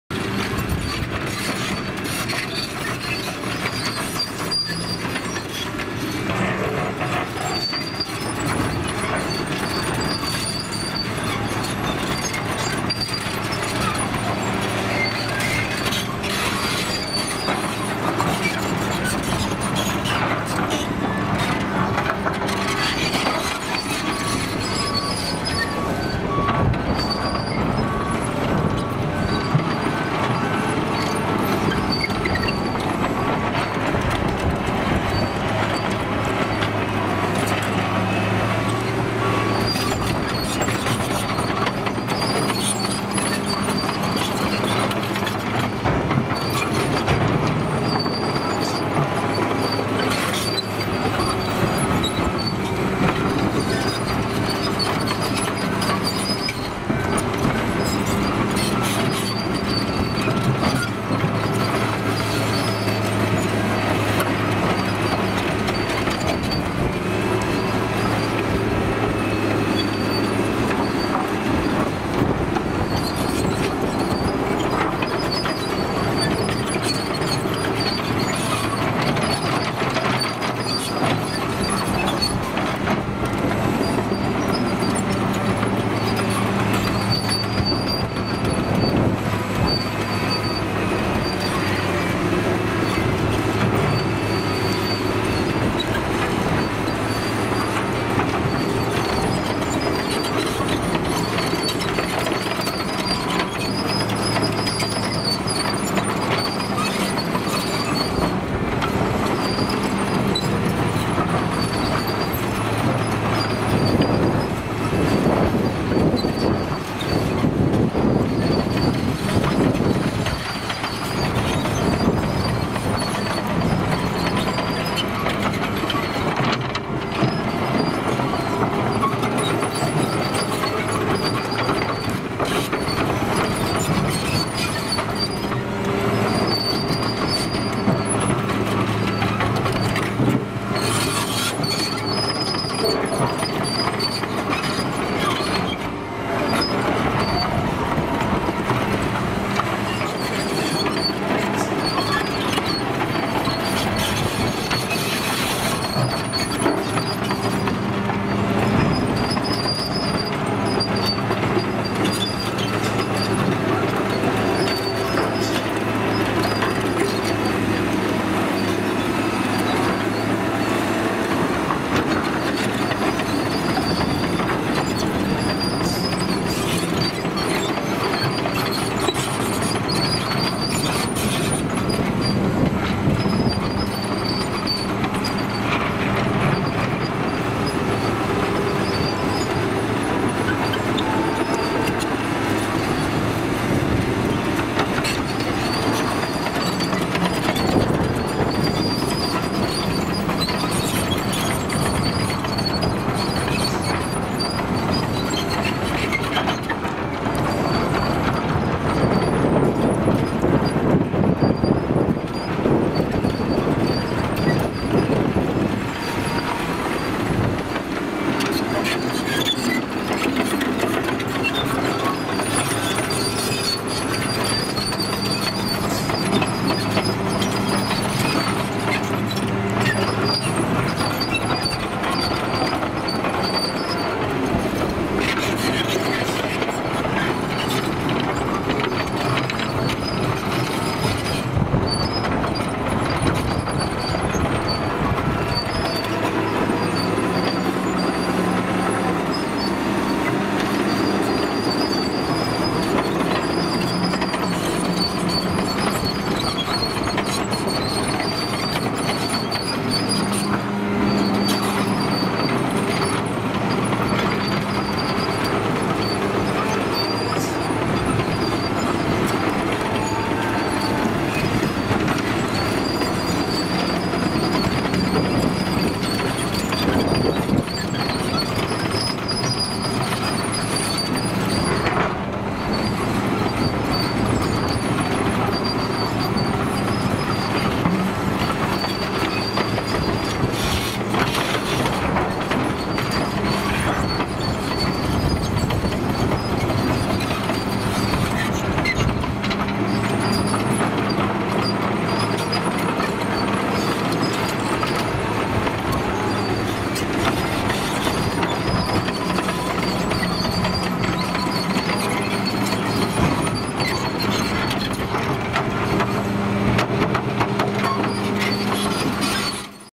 CAT 953C Track Loader Moving Dirt and Rock
All Seasons Excavating